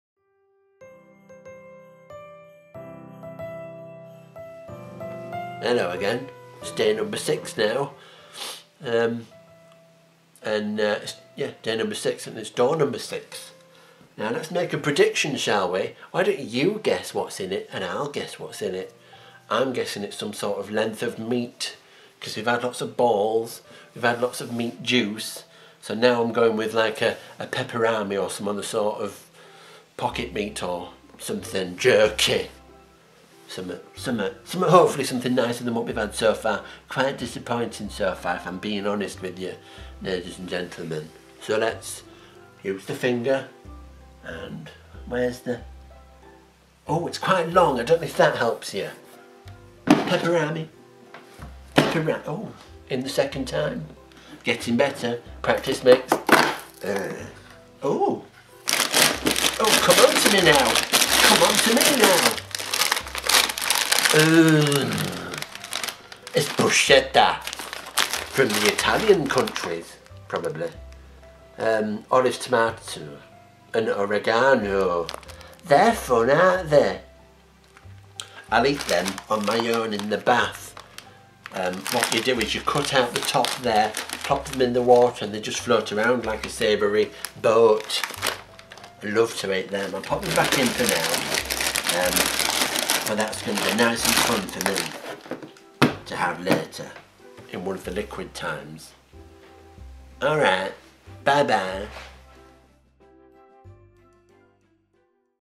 Day 6 - Degusta Advent Calendar 2022
Every day until christmas will open this advent calendar. What delights will be being each day? Watch and find out!